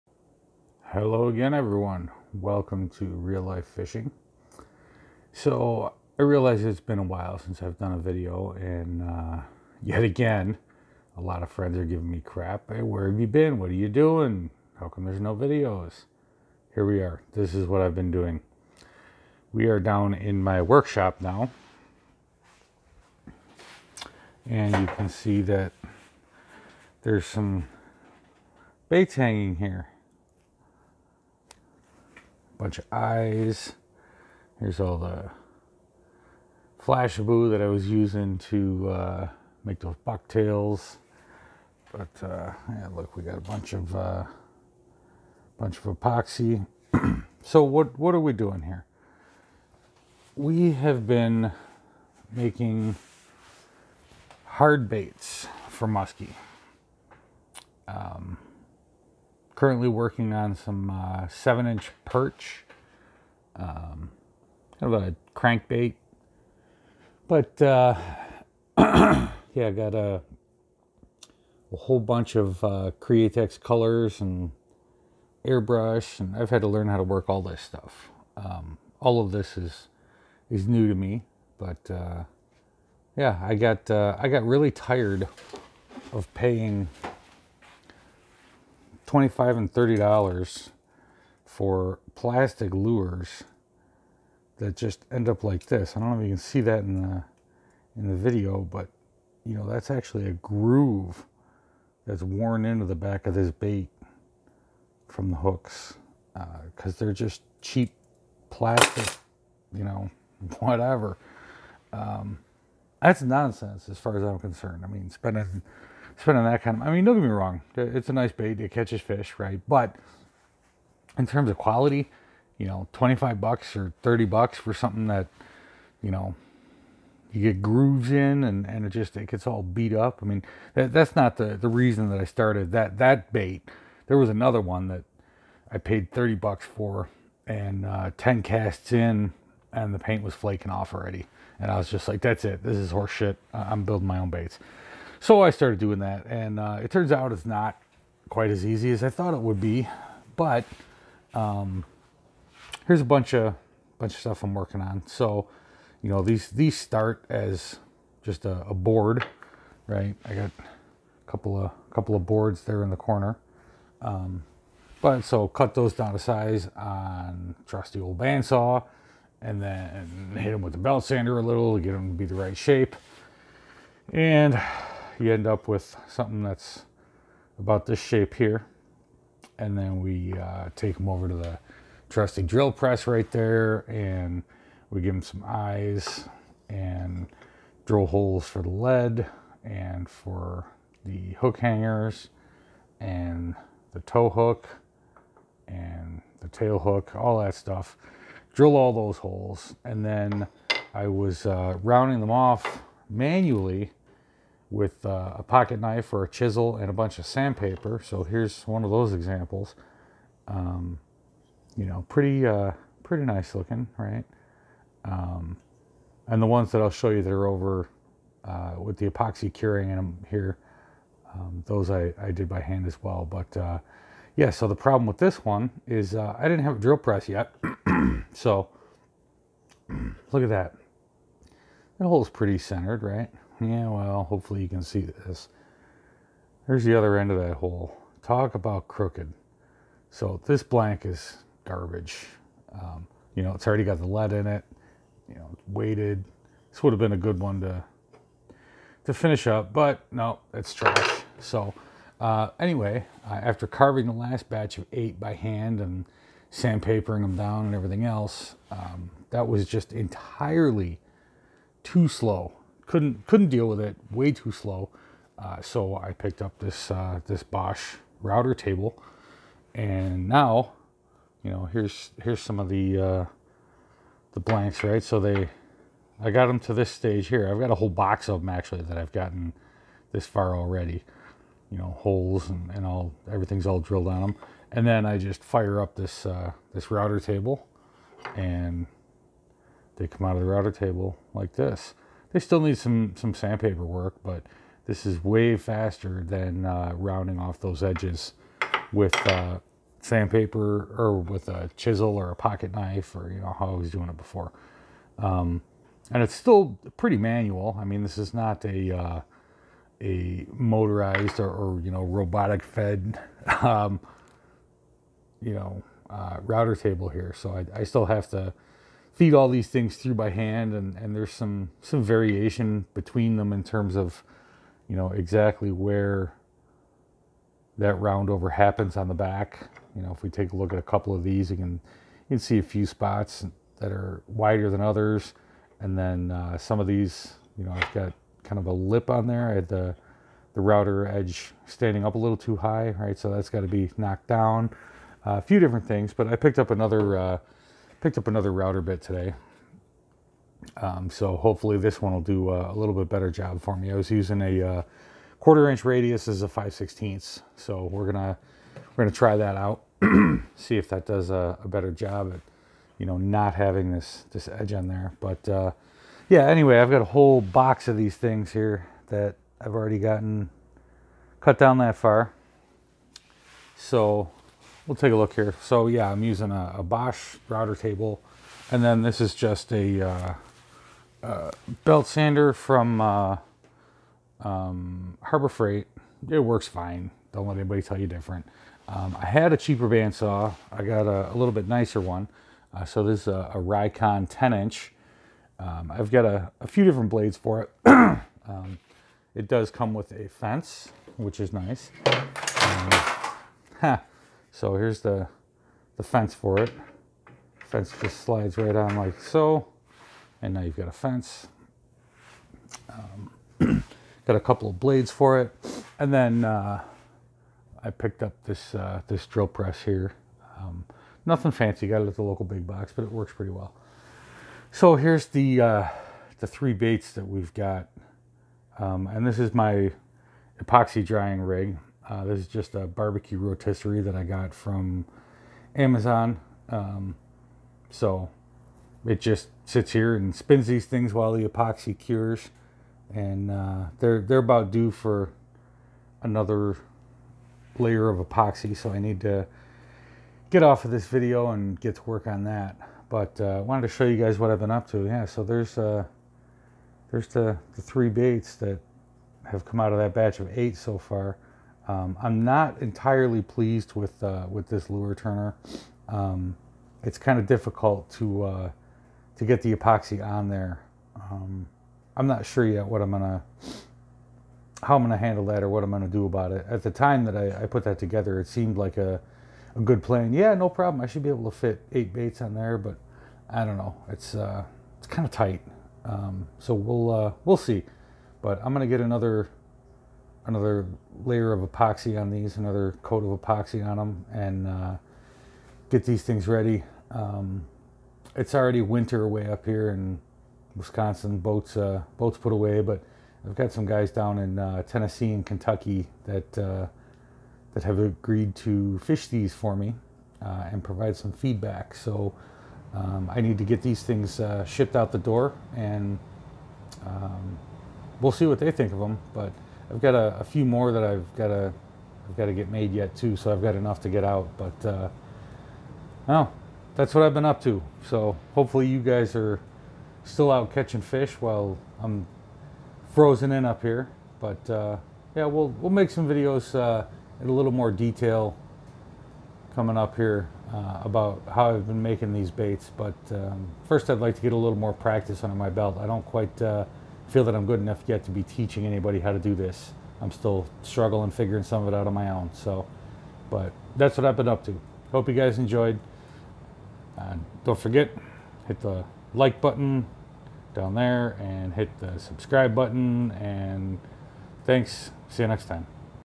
Where have I been? Making Musky lures!!
In this video, we take a look at my workshop for my latest project - making custom wooden Musky lures!